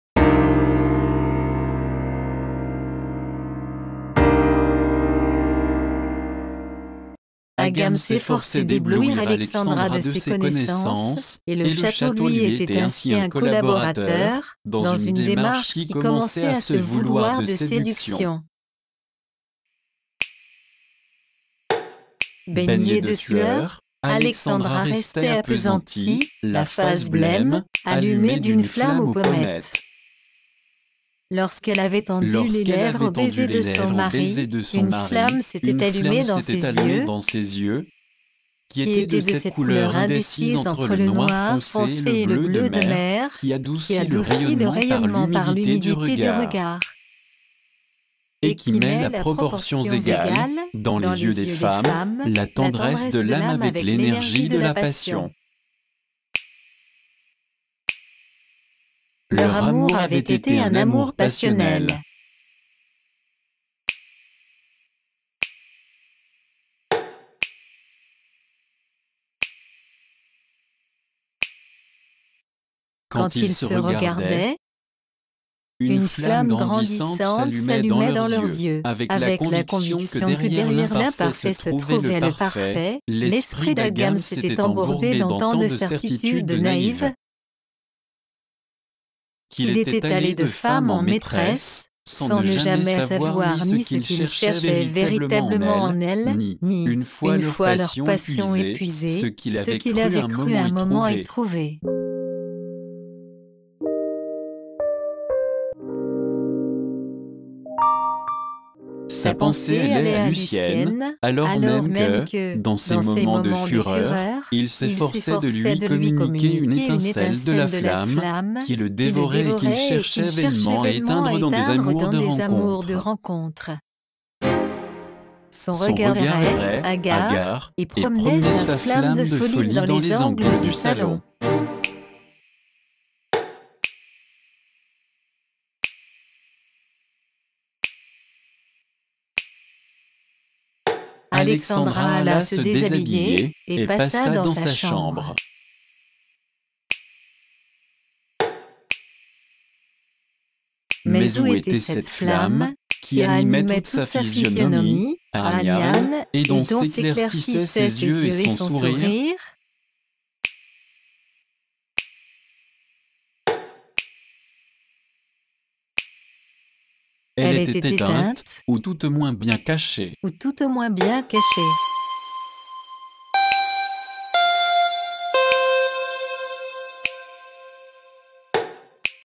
Cantate (Saison 1, Épisode 10)
Albertine Ganançay propose ici une deuxième version personnelle de la vidéosérie Cantate. Un texte automatique sur une citation d'Abel Gance et une musique de Pierre Charlus.